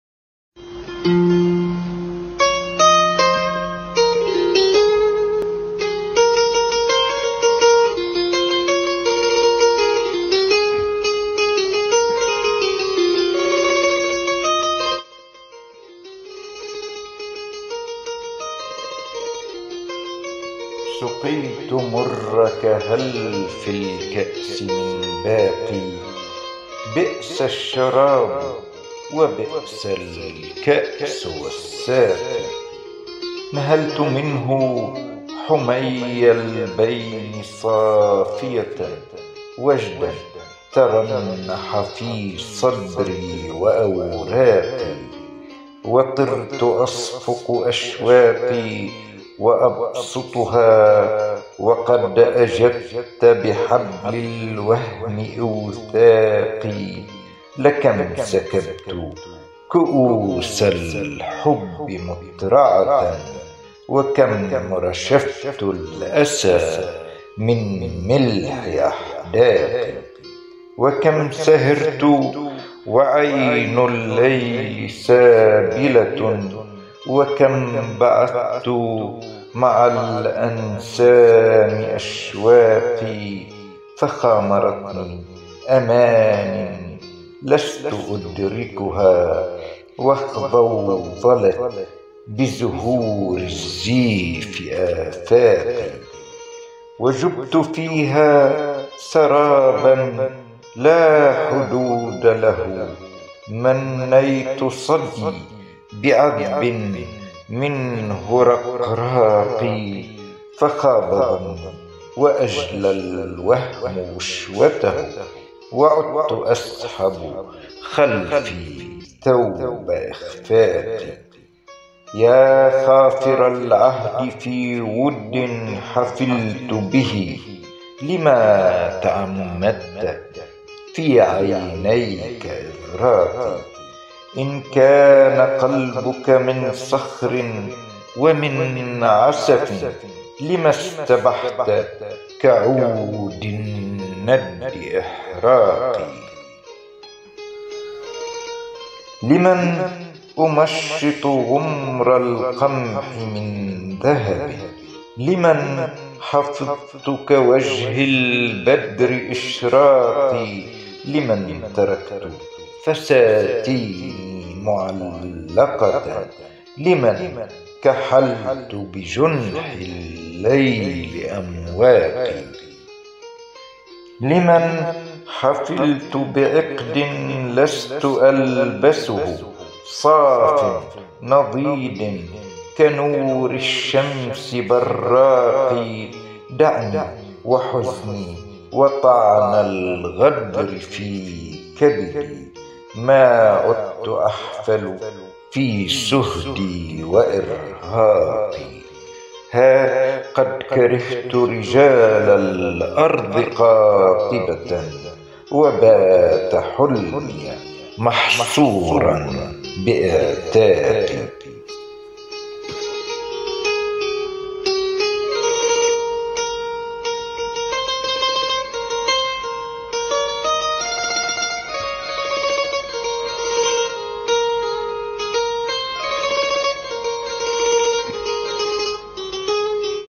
صرخة امرأة مغدورة: شعر وإلقاء: هاني زريفة
صرخةُ امرأةٍ مغدورةٍ
قصيدة علىٰ لسان امرأة لديوان (أوراق امرأة عاشقة).
شعر: هاني زريفة

١ ـ سُقِيْتُ مُرَّكَ هَلْ في الكأسِ مِنْ باقِ؟!
     بِئْسَ الشَّرابُ وبِئْسَ الكَأْسُ والسَّاقي!

٢ ـ نَهَلْتُ مِنْهُ حُمَيَّا البَيْنِ صافِيَةً
     وَجْداً تَرَنَّحَ في صَدْري وأَوراقي

٣ ـ وطِرتُ أَصْفُقُ أشواقي وأَبْسُطُها
     وقَدْ أَجَدْتَ بحبلِ الوَهْمِ إوْثاقي

٤ ـ لَكَمْ سَكَبْتُ كُؤوسَ الحُبِّ مُتْرَعَةً
     وكَمْ رَشَفْتُ الأَسىٰ مِنْ مِلْحِ أَحْداقي

٥ ـ وكَمْ سَهِرْتُ وعَينُ اللَّيلِ سابِلَةٌ
     وَكَمْ بَعَثْتُ مَعَ الأَنْسامِ أَشْواقي

٦ ـ فَخامَرَتْني أَمانٍ لَسْتُ أُدْرِكُها
     واخْضَوْضَلَتْ بِزُهُورِ الزِّيْفِ آفاقي

٧ ـ وجُبْتُ فيها سَراباً لا حُدودَ لَهُ
     مَنَّيْتُ صَدْيي بَعَذْبٍ مِنْهُ رَقْراقِ

٨ ـ فَخابَ ظَنِّي وأَجْلىٰ الوَهْمُ غُشْوَتَهُ
     وعُدْتُ أَسْحَبُ خَلْفي ثَوْبَ إِخْفاقي

٩ ـ يا خافِرَ العَهْدِ في وُدٍّ حَفِلْتُ بِهِ
     لِمَ تَعَمَّدْتَ في عَيْنَيْكَ إِغْراقي؟!

١٠ ـ إِنْ كانَ قَلبُكَ مِنْ صَخْرٍ ومِنْ عَسَفٍ
      لِمَ اسْتَبَحْتَ كَعودِ النَّدِ إِحْراقي؟!

١١ ـ لِمَنْ أُمَشِّطُ غُمْرَ القَمْحِ مِنْ ذَهَبٍ
      لِمَنْ حَفَظْتُ كَوَجْهِ البَدْرِ إِشْراقي؟!

١٢ ـ لِمَنْ تَرَكْتُ فَساتيني مُعَلَّقَةً
      لِمَنْ كَحَلْتُ بِجُنْحِ اللِّيْلِ أَمْواقي؟!

١٣ ـ لِمَنْ حَفِلْتُ بِعِقْدٍ لَسْتُ أَلْبَسُهُ
      صافٍ نَضيدٍ كَنورِ الشَّمْسِ بَرّاقِ؟!

١٤ ـ دَعْني وحُزني وطَعْنَ الغَدْرِ في كَبِدي
      ما عُدْتُ أحفَلُ في سُهْدي وإرْهاقي

١٥ ـ ها قَدْ كَرِهْتُ رِجالَ الأَرْضِ قاطِبَةً
      وباتَ حُلْمِيَ مَحْصوراً بإعْتاقي